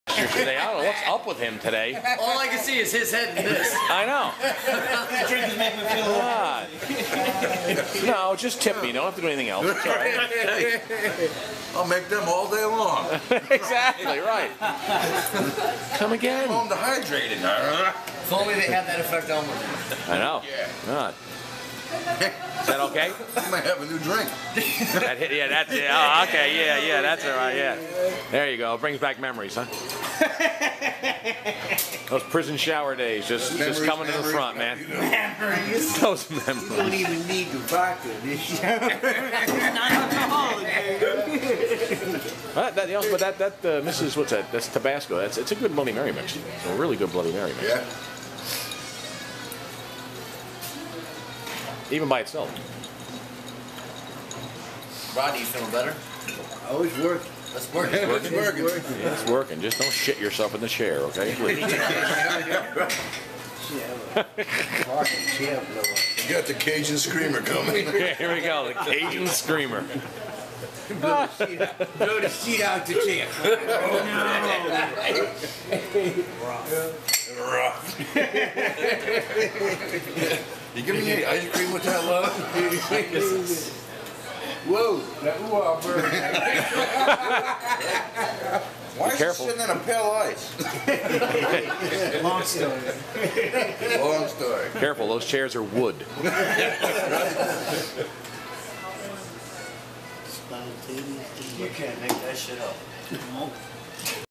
CAJUN SCREAMER @ THE WHEREHOUSE NEWBURGH NEW YORK 01-28-2012
Rodni explains spicy food and its after effects